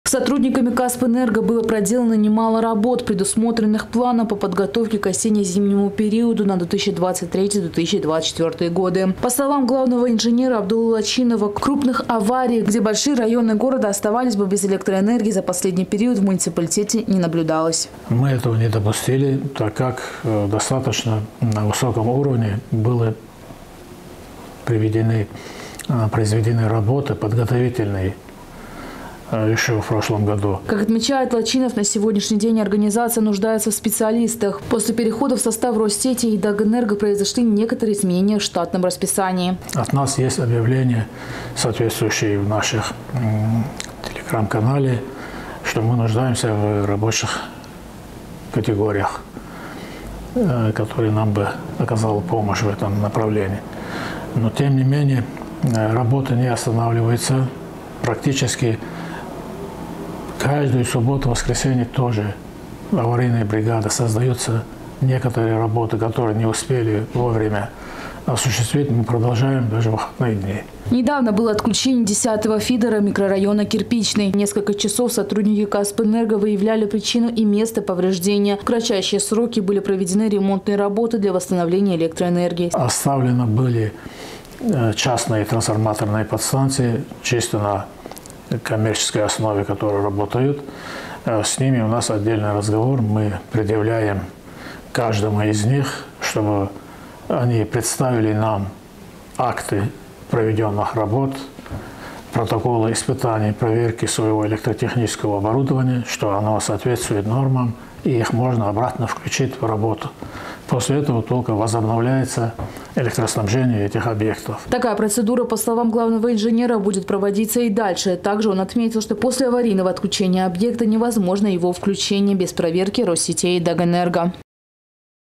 Главный инженер «Каспэнерго» рассказал о работе организации
Сотрудниками «Каспэнерго» было проделано немало работ, предусмотренных планом по подготовке к осенне-зимнему периоду  на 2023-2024 годы. По словам главного инженера Абдула Лачинова, крупных аварий, где большие районы города оставались бы без электроэнергии за последний период в муниципалитете не наблюдалось.

Как отмечет Лачинов, на сегодняшний день организация нуждается в специалистах. После перехода в состав «Россетей» и «Дагэнерго» произошли некоторые изменения в штатном расписании.